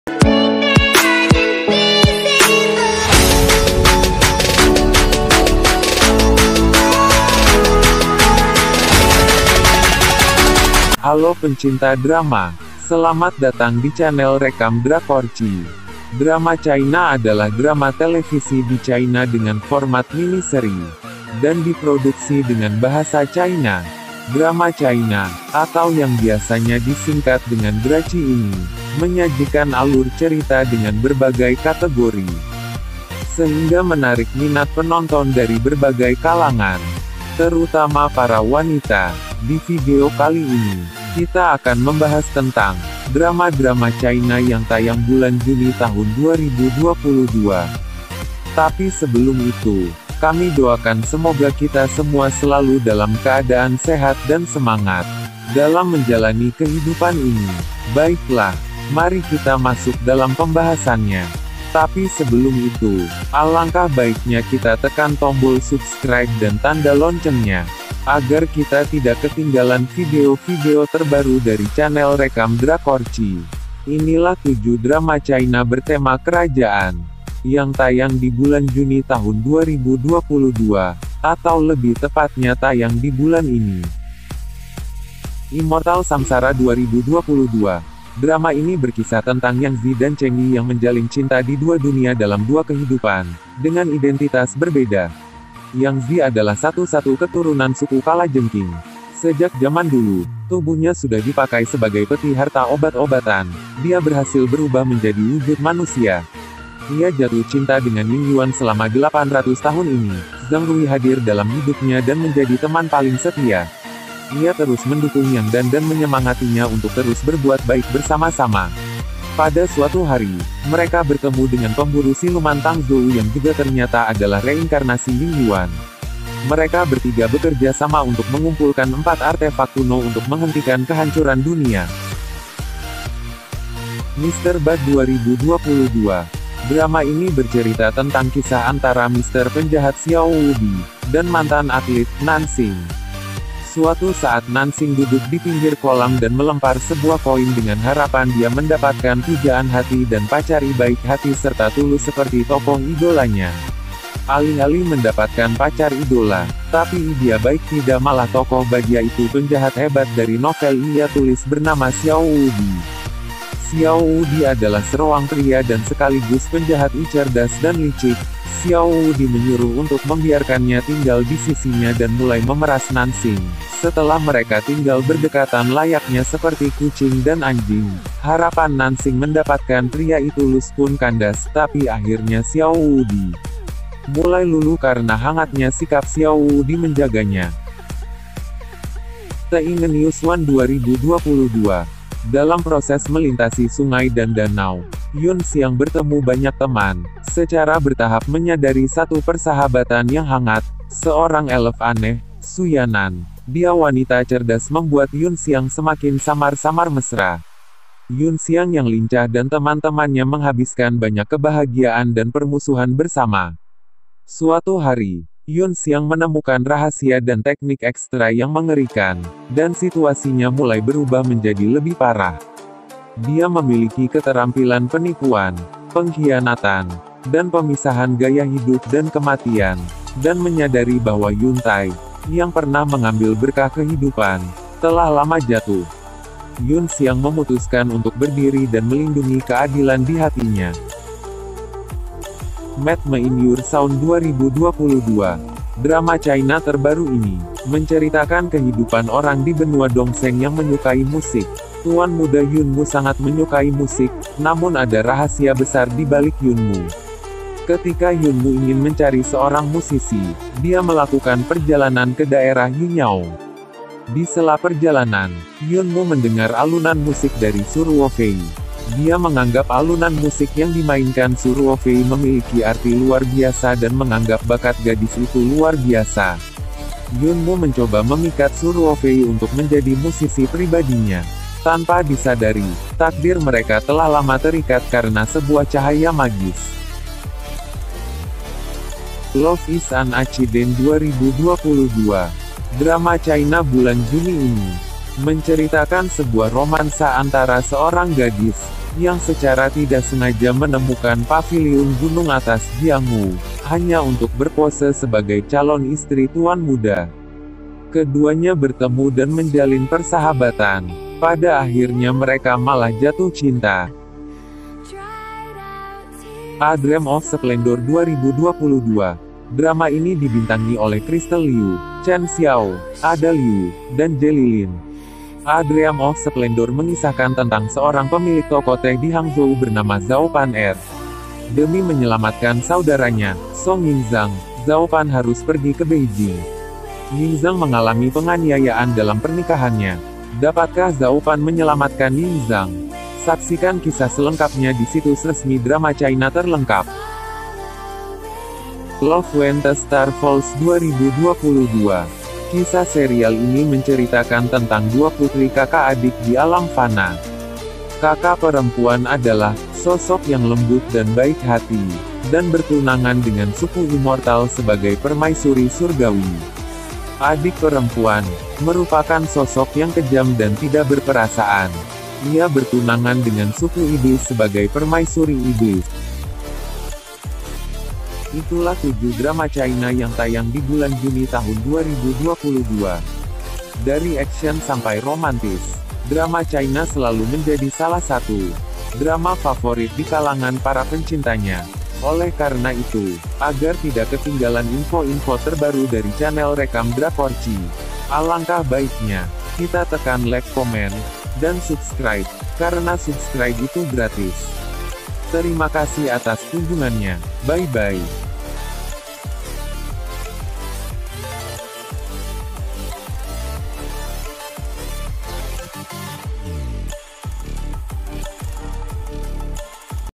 7 Drama china kerajaan terbaru yang tayang juni 2022
Di video kali ini kita akan menampilkan 7 Drama china kerajaan terbaru yang tayang juni 2022 yang terpopuler di tahun ini.